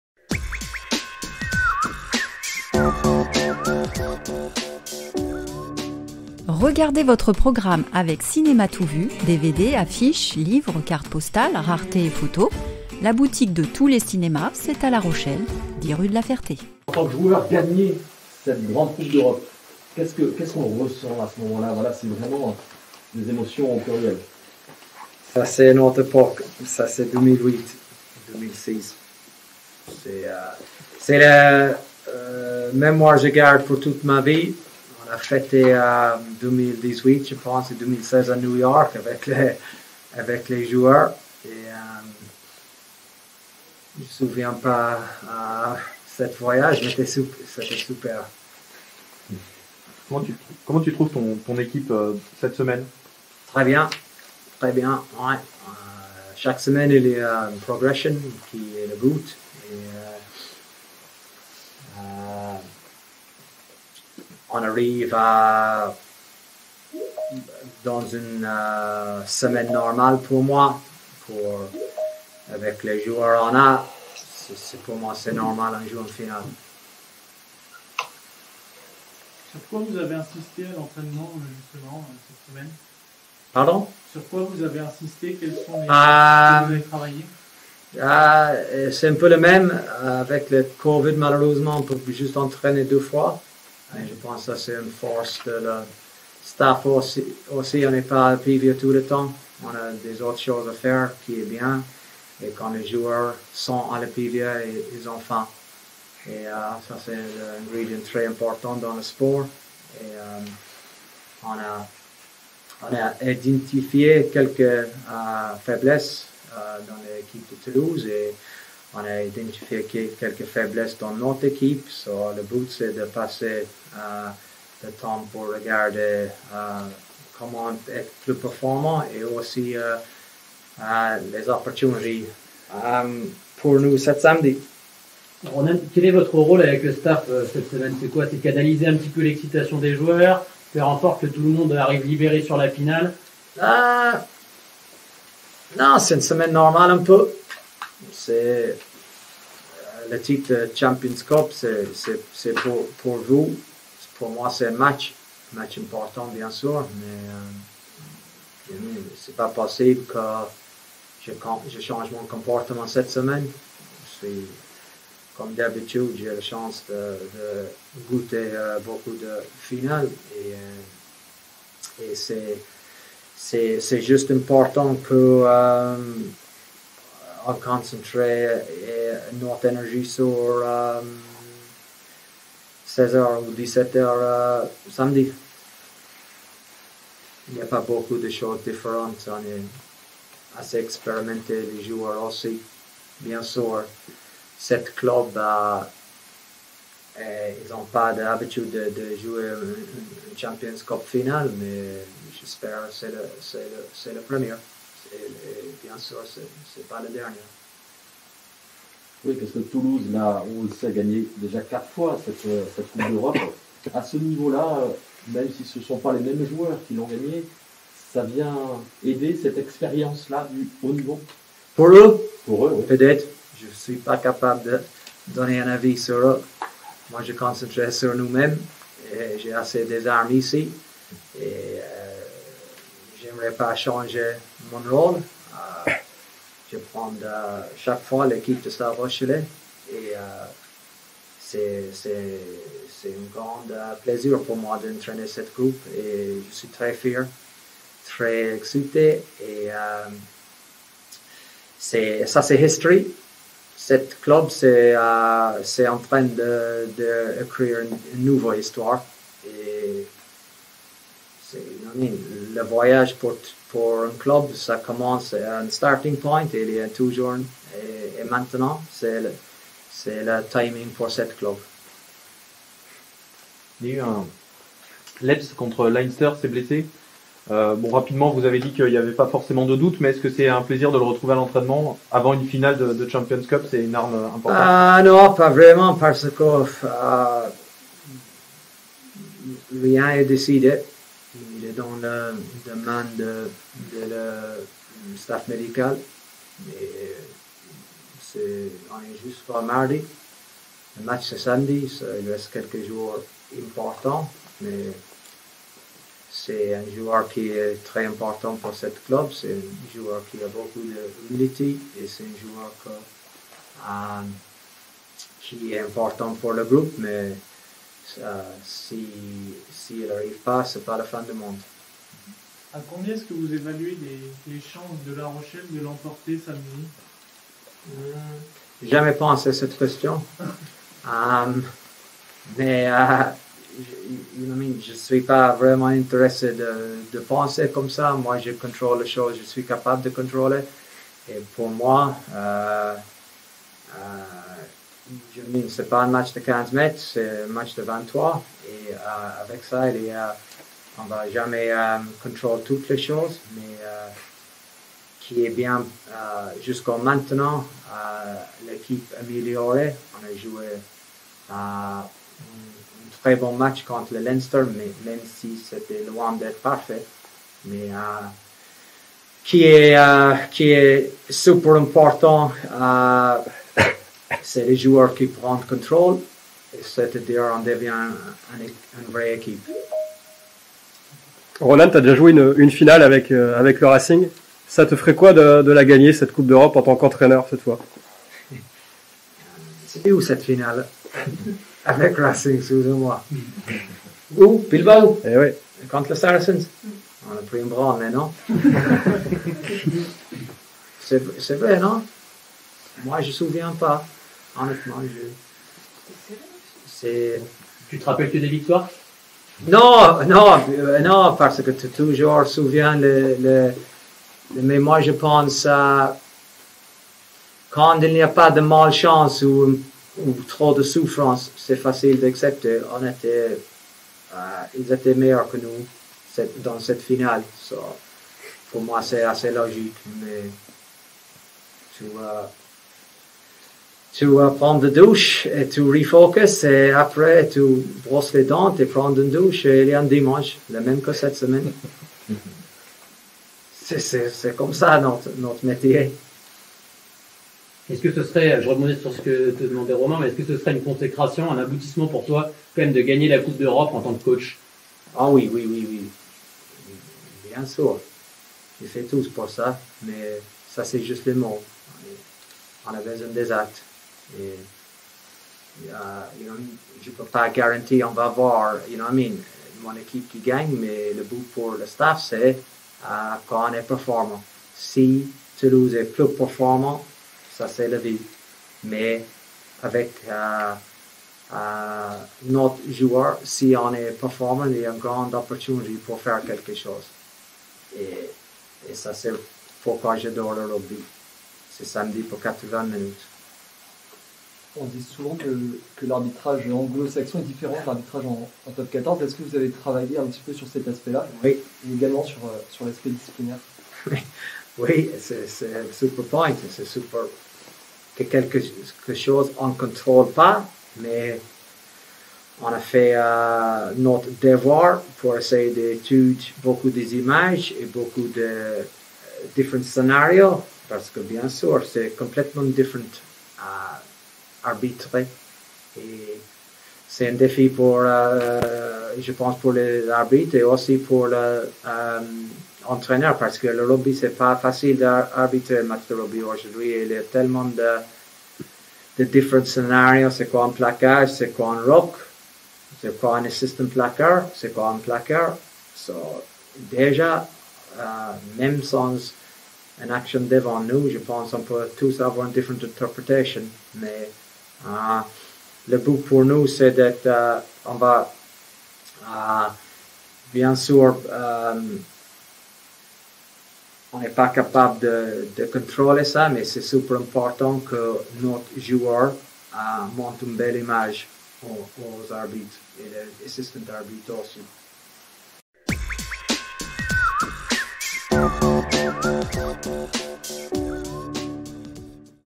La Rochelle Conférence De Presse D'avant Match Champions Cup Avec Ronan O'gara
L'effervescence monte crescendo depuis le début de semaine dans les rues de La Rochelle, du côté des joueurs aussi, en pleine préparation de cette finale historique de Champions Cup contre le Stade Toulousain.